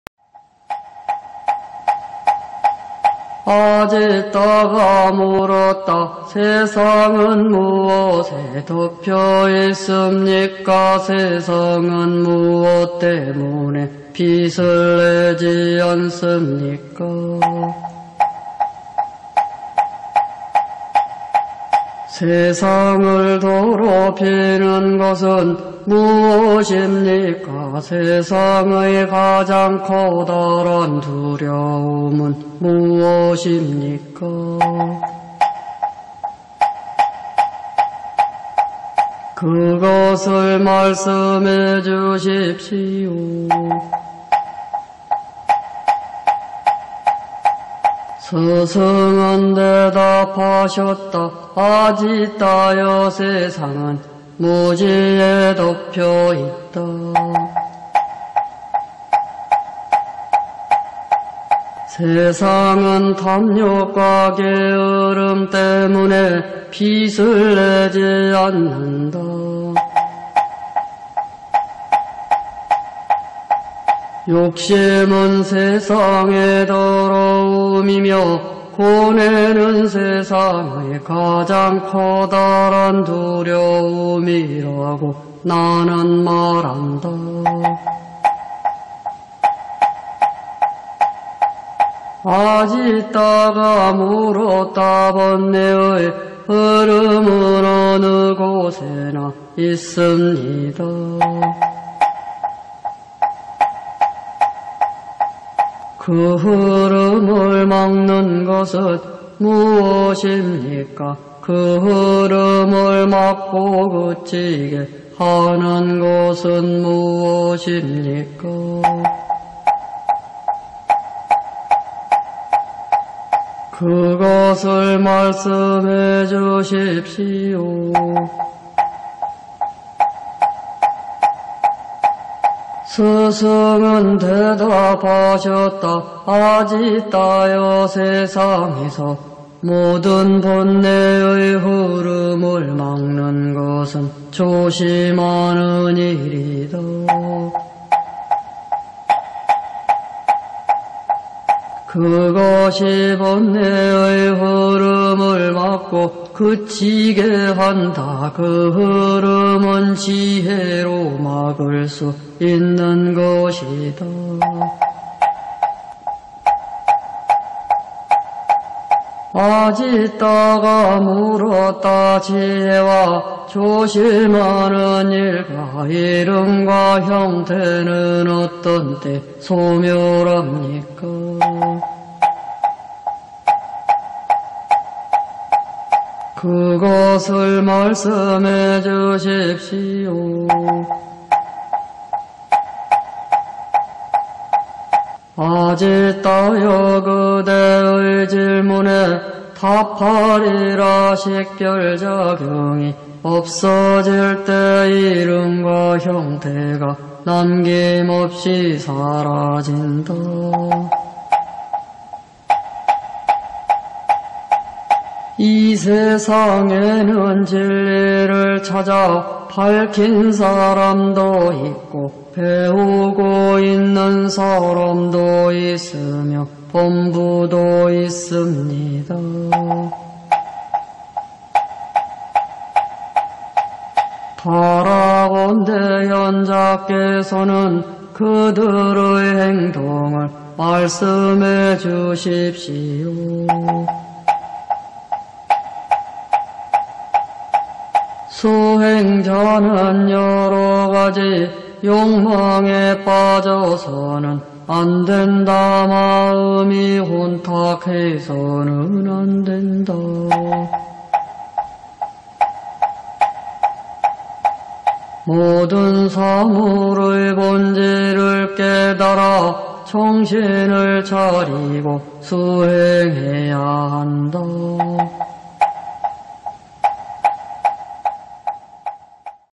56.아지따의 질문 經 [숫따니빠따 제5장中 2]
56. 아지따(=아지타)의 질문 經

The Suttanipata 제 5 [ 피안에 이르는 길 ]

오십육경 72經中 오십육經[2. 아지따의 질문經 ]

2) 아지따의 질문 經/ 348 [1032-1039]

1032

아지따가 물었다. "세상은 무엇에 덮여 있습니까. 세상은 무엇 때문에 빛을 내지 않습니까.

세상을 더럽히는 것은 무엇입니까. 세상의 가장 커다란 두려움은 무엇입니까? 그것을 말씀해 주십시오."

1033

스승은 대답하셨다. "아지따여, 세상은 무지에 덮여 있다. 세상은 탐욕과 게으름 때문에 빛을 내지 않는다.

욕심은 세상의 더러움이며, 고뇌는 세상의 가장 커다란 두려움이라고 나는 말한다."

1034

아지따가 물었다. "번뇌의 흐름은 어느 곳에나 있습니다. 그 흐름을 막는 것은 무엇입니까.

그 흐름을 막고 그치게 하는 것은 무엇입니까. 그것을 말씀해 주십시오."

1035

스승은 대답하셨다. "아지따여, 세상에서 모든 번뇌의 흐름을 막는 것은 조심하는 일이다.

그것이 번뇌의 흐름을 막고 그치게 한다. 그 흐름은 지혜로 막을 수 있는 것이다."

1036

아지따가 물었다. "지혜와 조심하는 일과 이름과 형태는 어떤 때 소멸합니까? 그것을 말씀해 주십시오."

1037

"아지따여, 그대의 질문에 답하리라. 식별 작용이 없어질 때 이름과 형태가 남김없이 사라진다."

1038

" 이 세상에는 진리를 찾아 밝힌 사람도 있고, 배우고 있는 사람도 있으며, 범부도 있습니다.

바라건대, 현자께서는 그들의 행동을 말씀해 주십시오."

1039

"수행자는 여러 가지 욕망에 빠져서는 안 된다. 마음이 혼탁해서는 안 된다.

모든 사물의 본질을 깨달아 정신을 차리고 수행해야 한다."